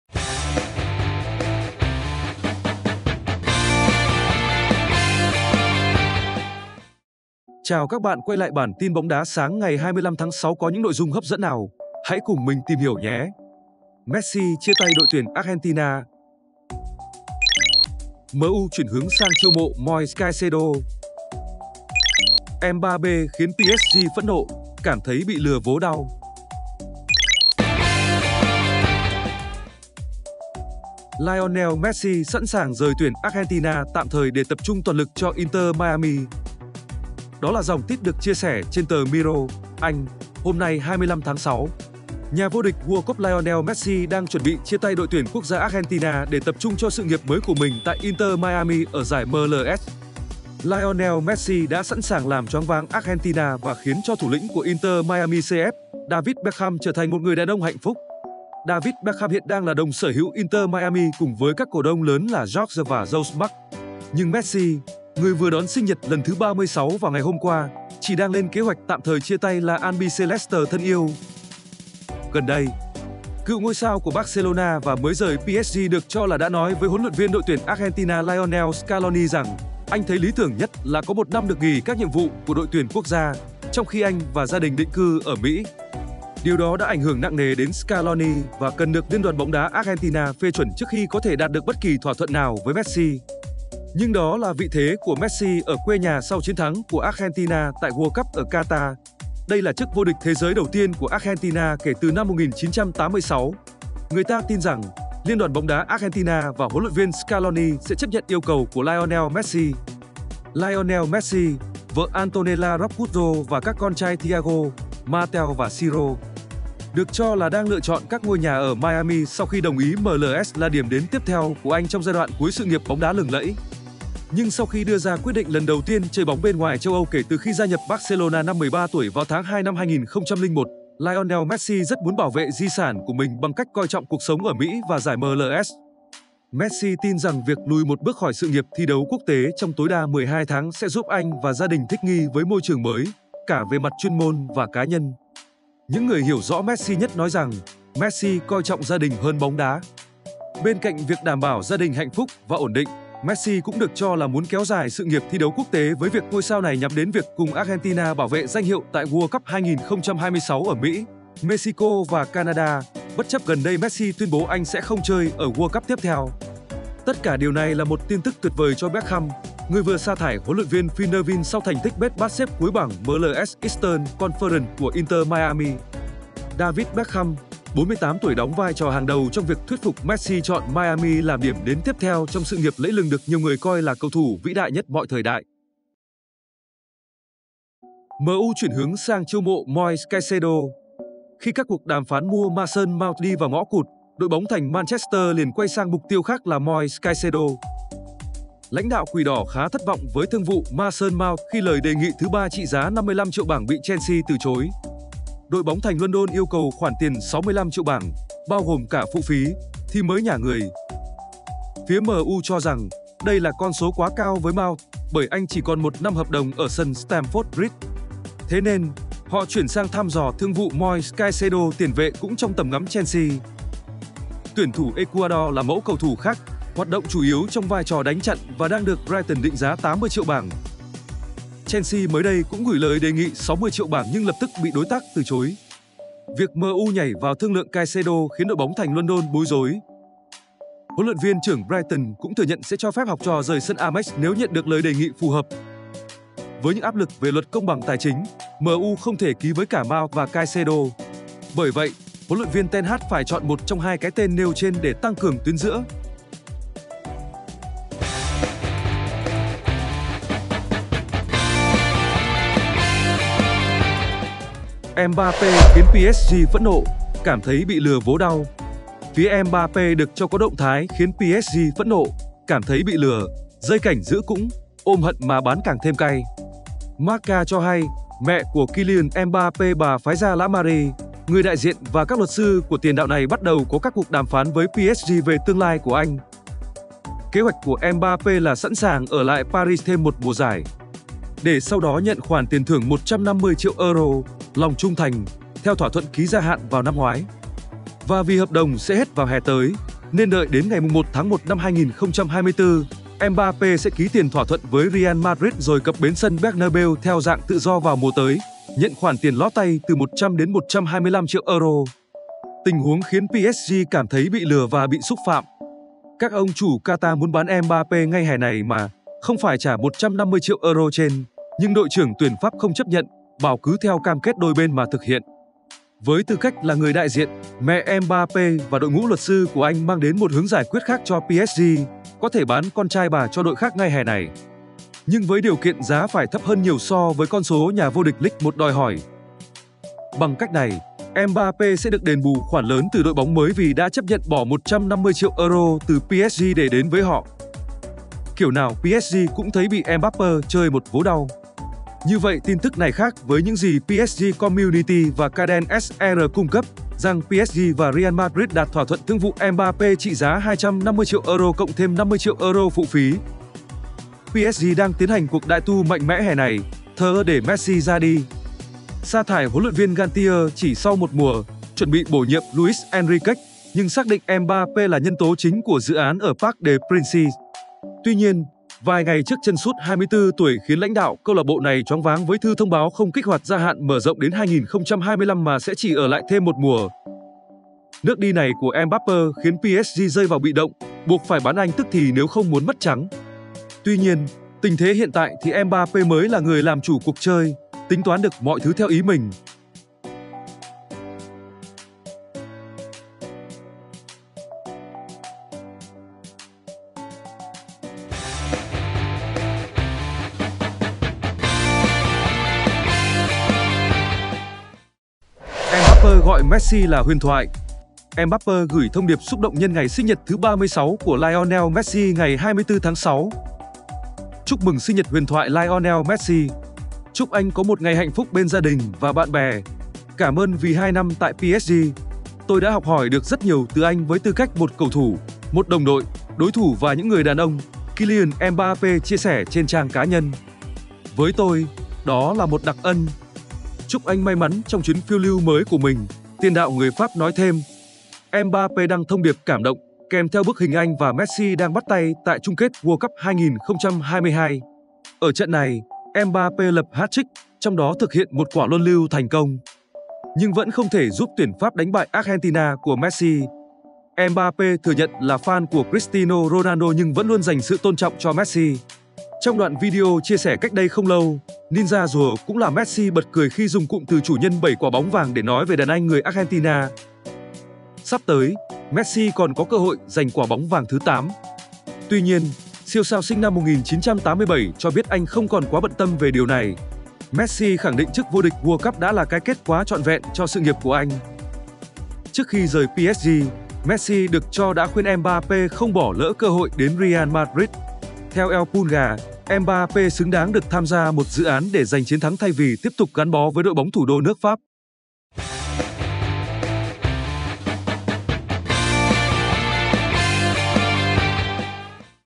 TIN BÓNG ĐÁ 25/6: Messi chia tay đội tuyển Argentina, Mbappe khiến PSG phẫn nộ - Spress TV
TIN BÓNG ĐÁ 25/6: Messi chia tay đội tuyển Argentina, Mbappe khiến PSG phẫn nộ
Chào các bạn quay lại bản Tin bóng đá sáng ngày 25/06 có những nội dung hấp dẫn nào, hãy cùng mình tìm hiểu nhé.
Messi chia tay đội tuyển Argentina
MU chuyển hướng sang chiêu mộ Moises Caicedo
Mbappe khiến PSG phẫn nộ, cảm thấy bị lừa vố đau

Lionel Messi sẵn sàng rời tuyển Argentina tạm thời để tập trung toàn lực cho Inter Miami.